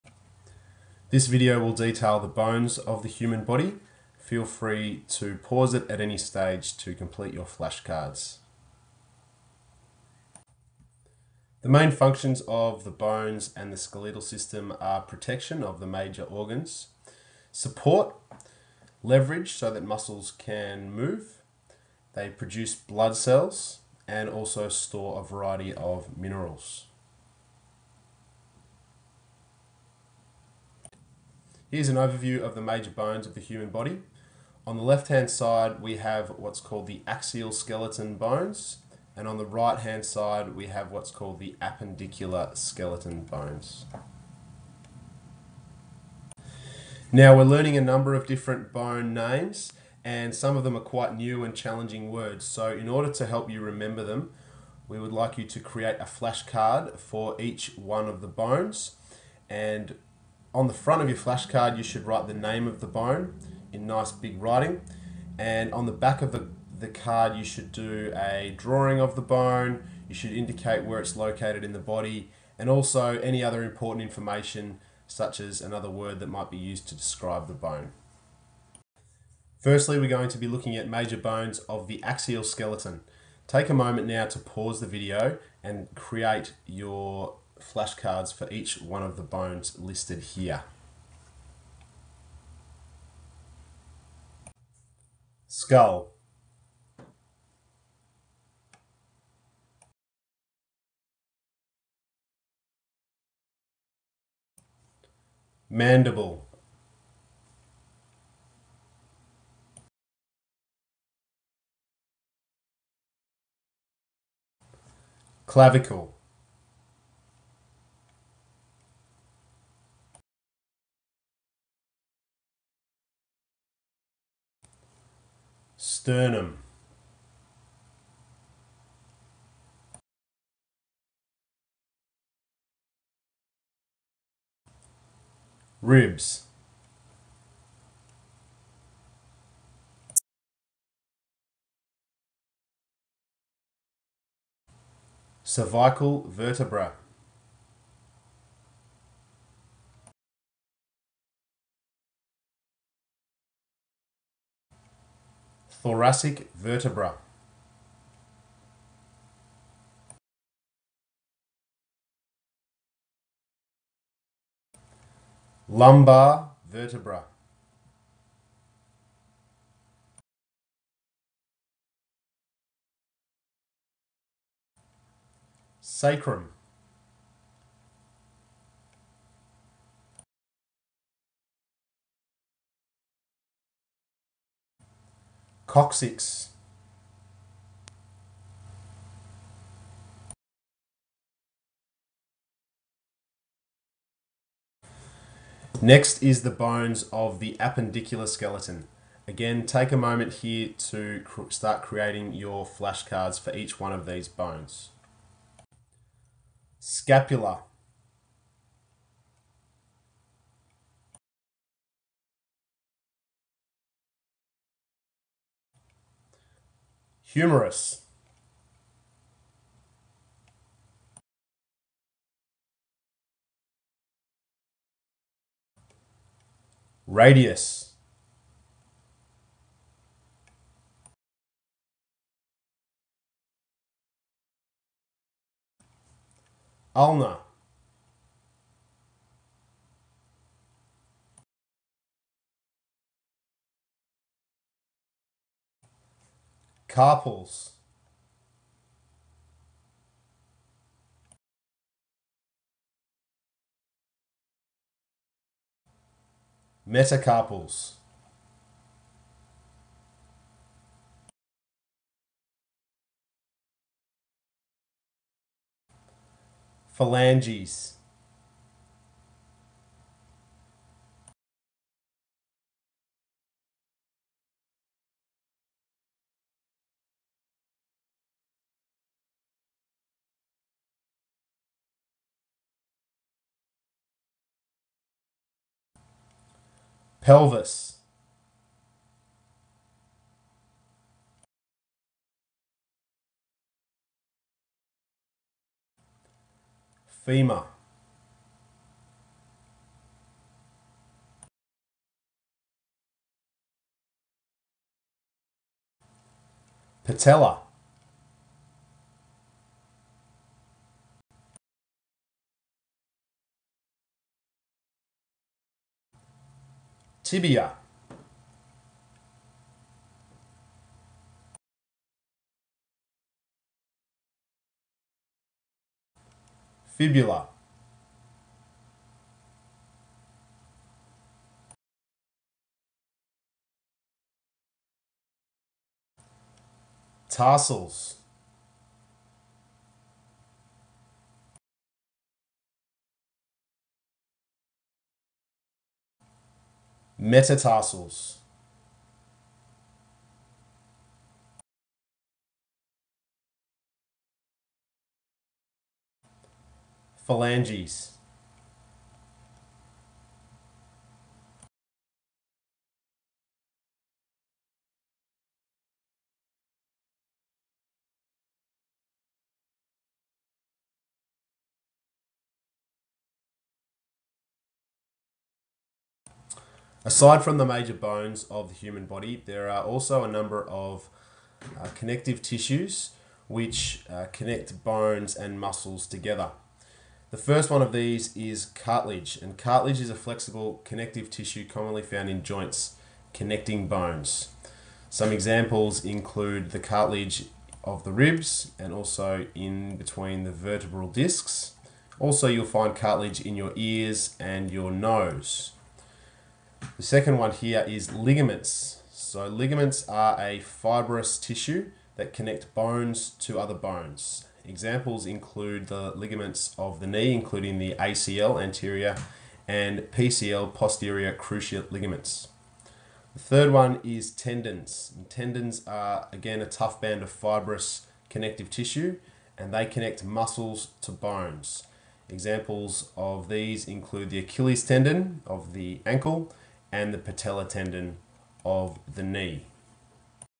Bones of the Human Body
An overview of the functions and names of major bones of the human body including axial and appendicular skeleton and connective tissue